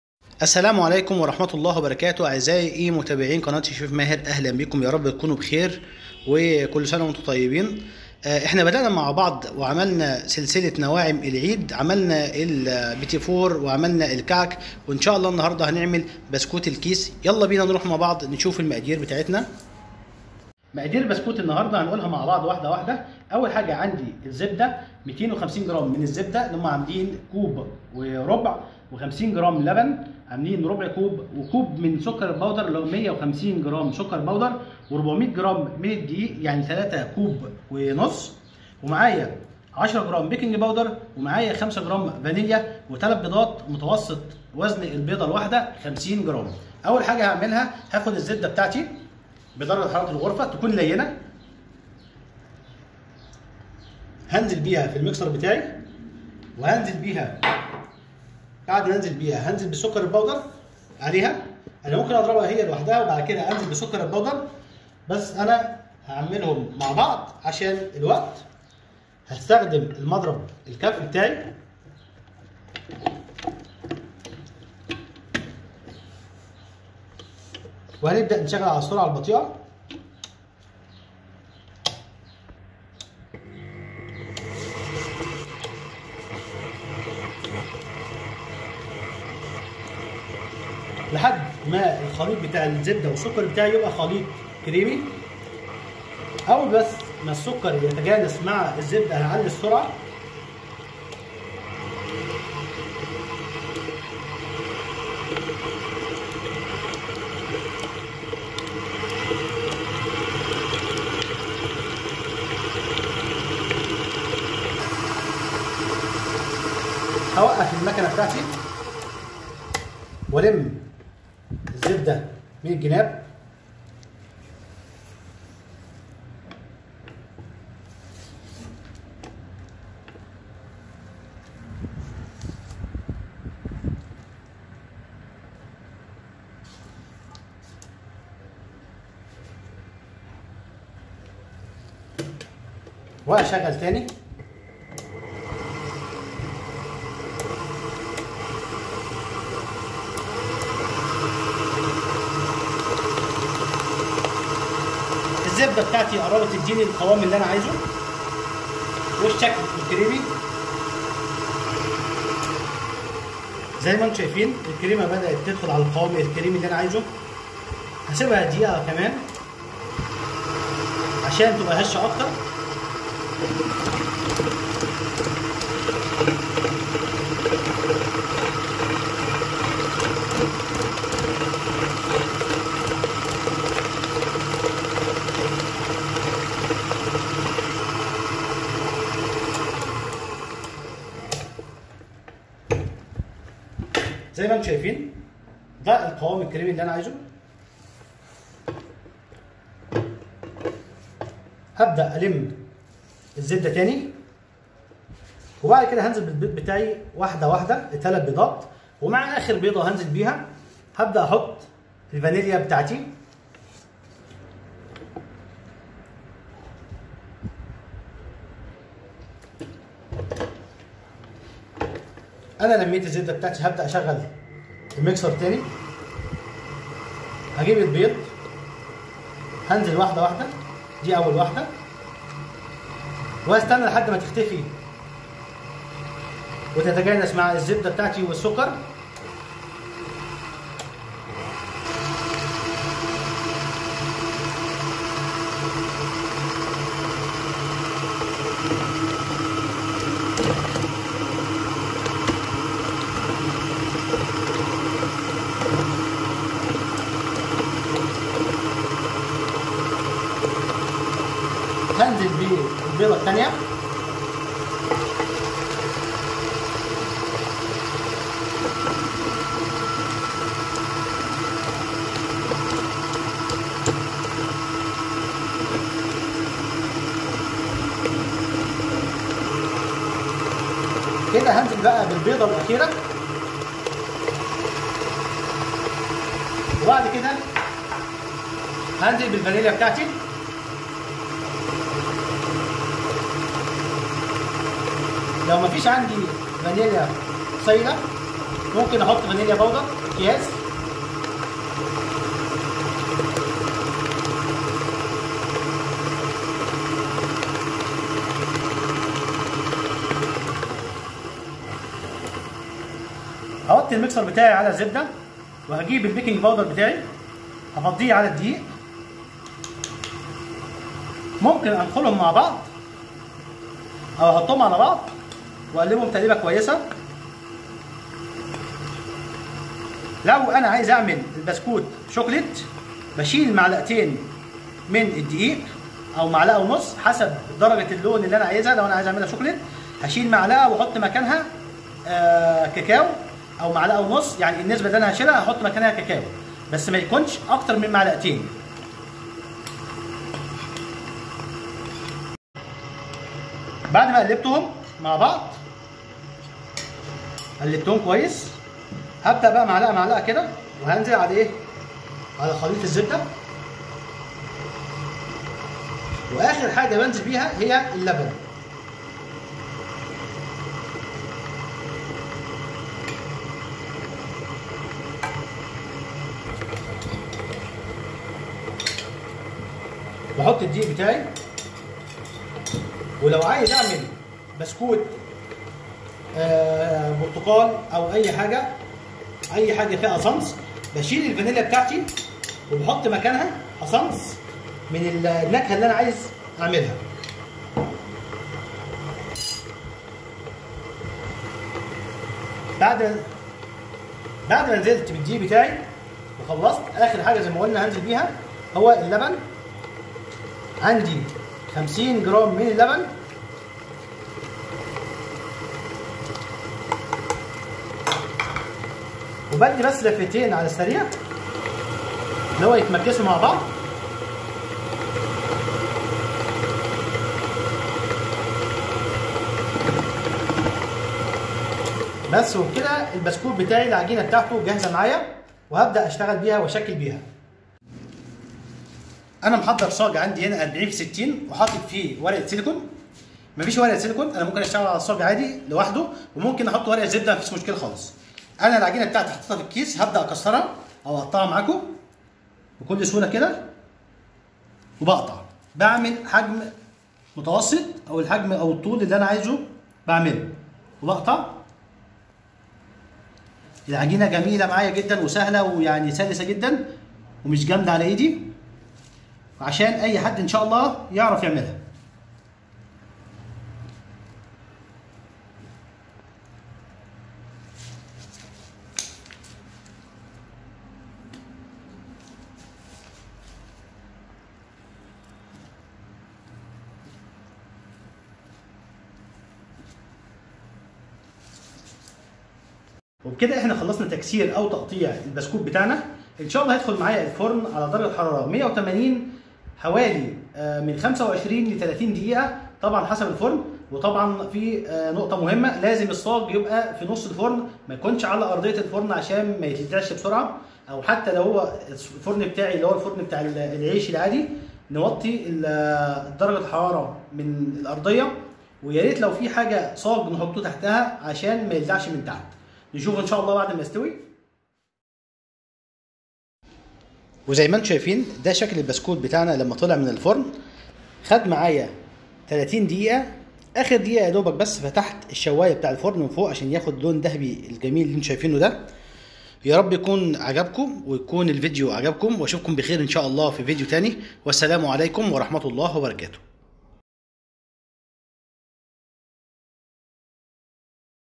بسكوت الكيس الناعم بدون نشادر جميل جدا وسهل جدا ومش هاتبطلو تعملوه
العجينة ناعمة وطرية ومش هاتتعب ايدك وانتي شغالة
يلا نشوف مقاديرنا

المكونات
 زبدة  ٢٥٠ جرام  ( كوب وربع ) سقعة ولينة طرية
سكر بودر  ١٥٠ جرام ( ١ كوب )
بيض  عدد ٣
فانيليا  ٥ جرام
دقيق  ٤٠٠ جرام  ( ٣ كوب ونص )
بيكنج بودر ١٠ جرام
لبن ٥٠ جرام  ربع كوب )
لعمل  بسكوت كيس شوكلت
نشيل ملعقة ونص دقيق ونضيف بدلهم كاكاو
لعمل بسكوت كيس برتقال
نشيل الفانيليا ونضيف بدلها اسانس برتقال .
لو من غير اسانس يبقي نشيل اللبن ونضيف مكانه عصير برتقال
ولكم جزيل الشكر
شيف ماهر